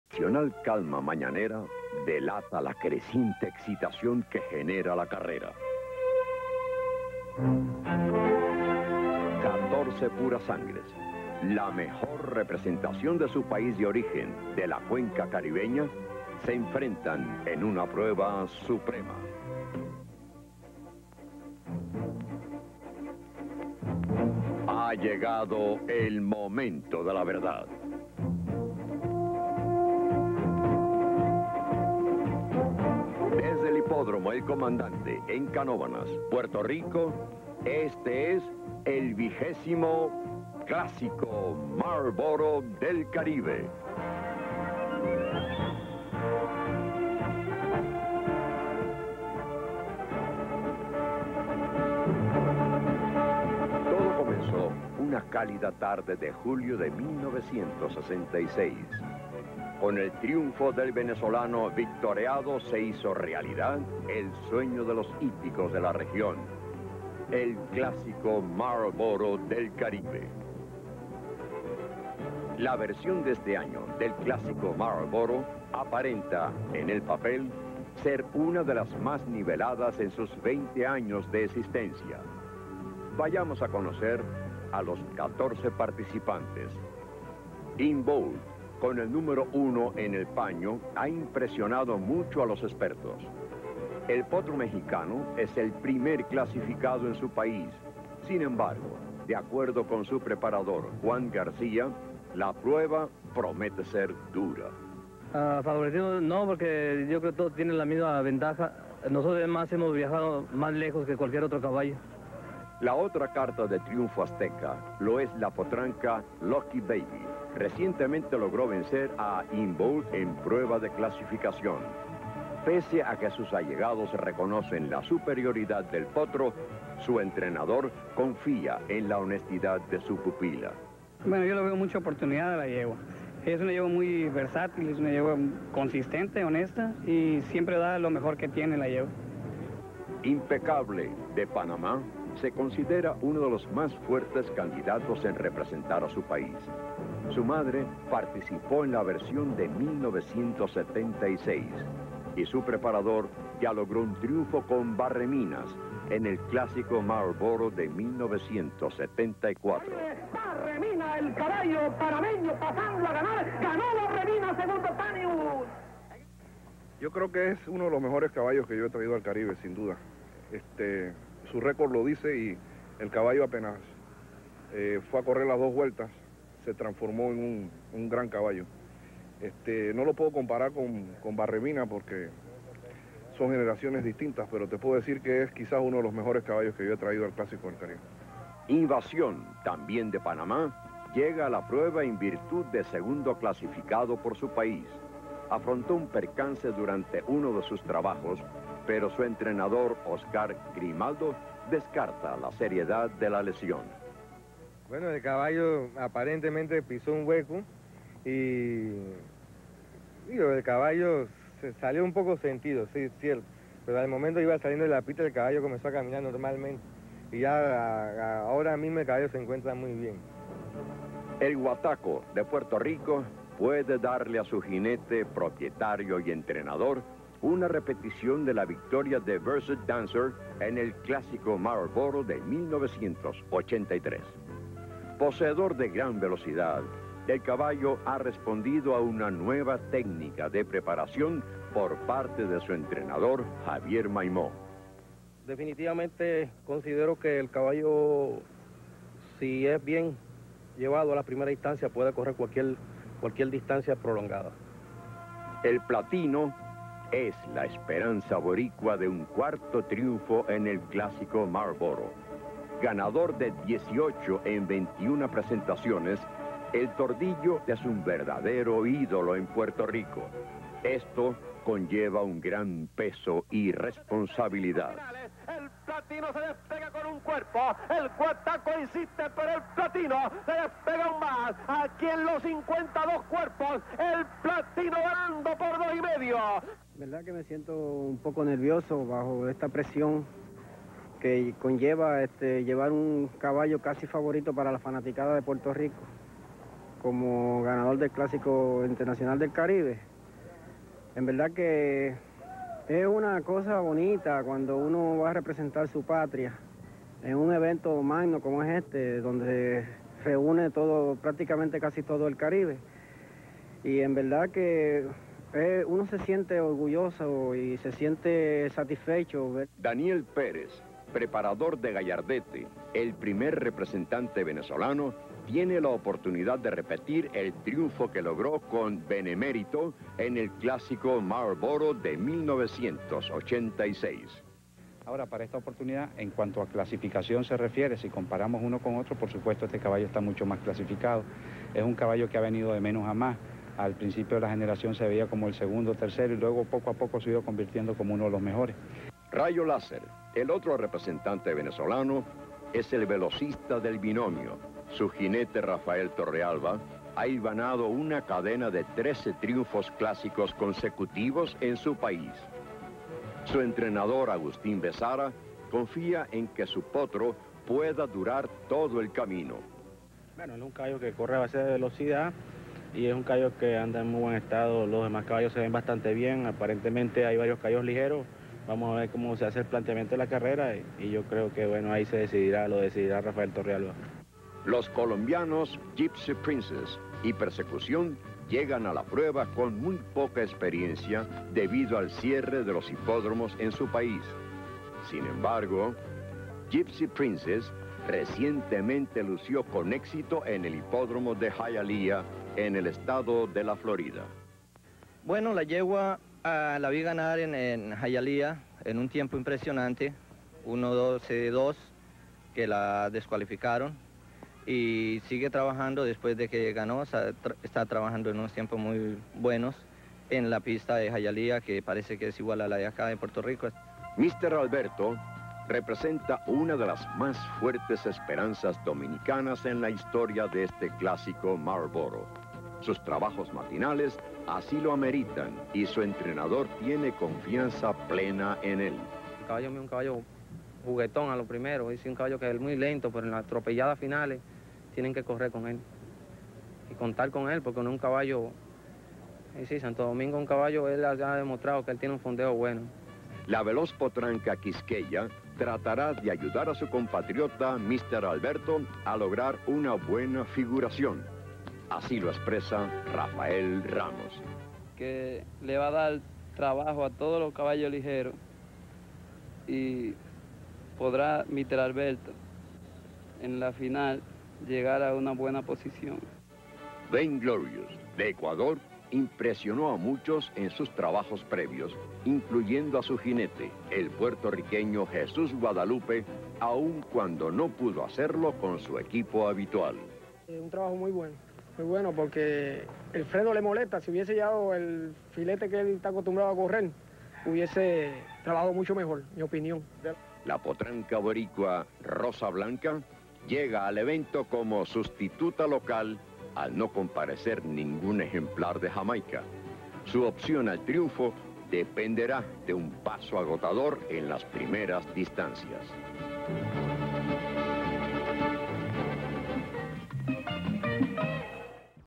REPORTAJE CLASICO DEL CARIBE 1987
Reportaje sobre cada uno de los participantes de la Vigésima edición del Clásico Internacional Del Caribe, celebrado en el hipódromo El Comandante, de Canóvanas, Puerto Rico, el domingo 6 de diciembre de 1987.
La prueba fue ganada por el venezolano Rayo Láser.